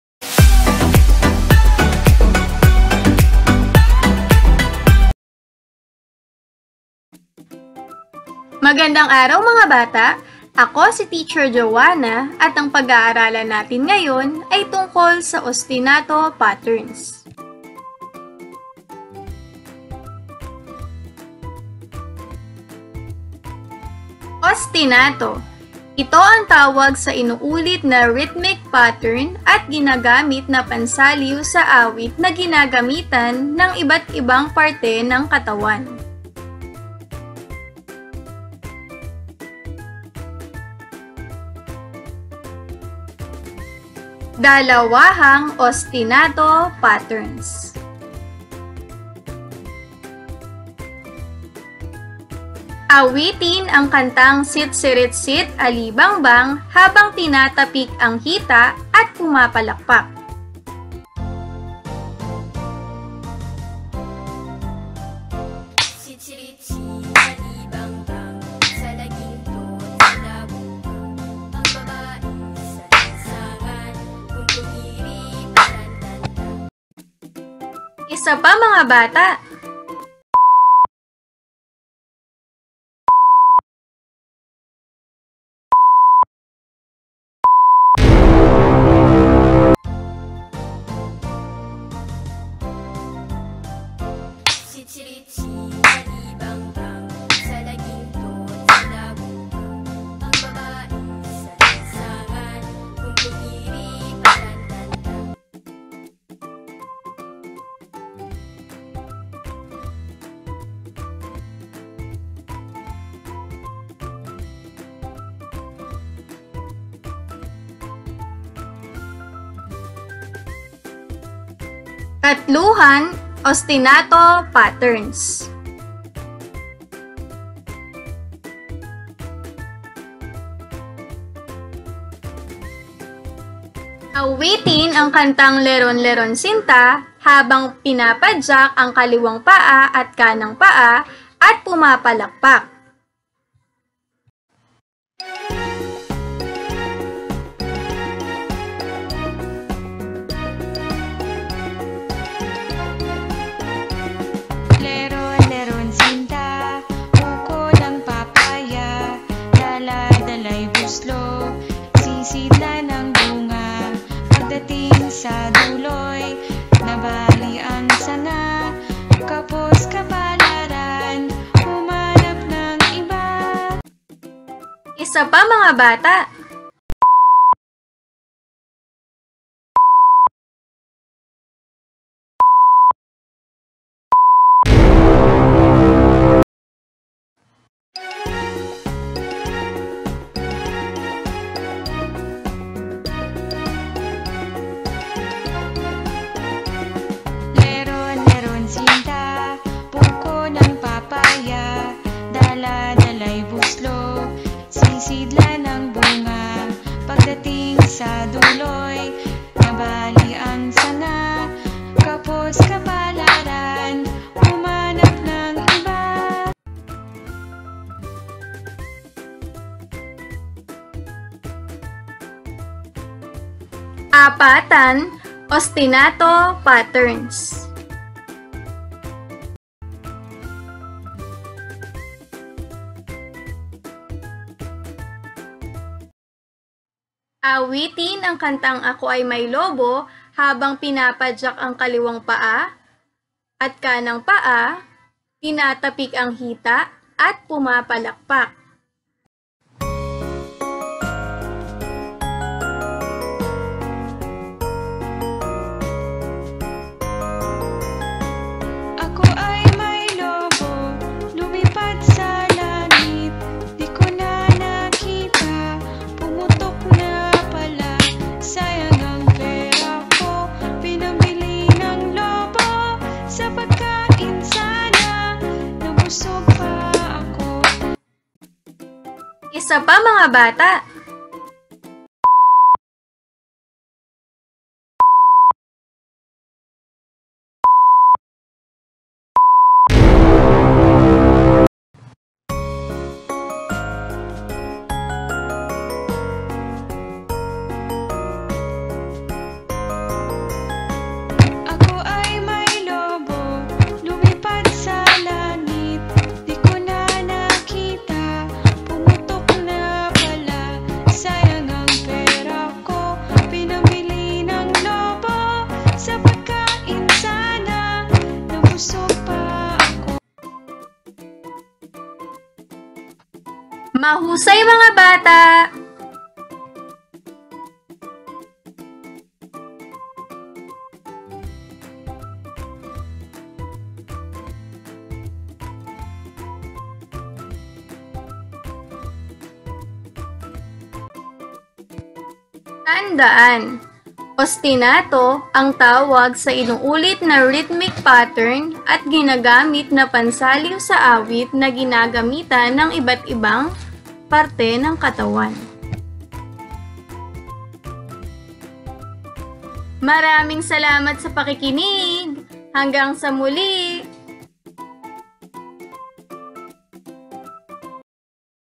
Ostinato Patterns- Music 1 Qtr 1 Week 5
Instructional Video by Ms. Joanna Rose B. Del Rosario, Teacher I of Juliano C. Brosas Elementary School
MUSIC 1 QTR 1 WEEK 5 MELC 4_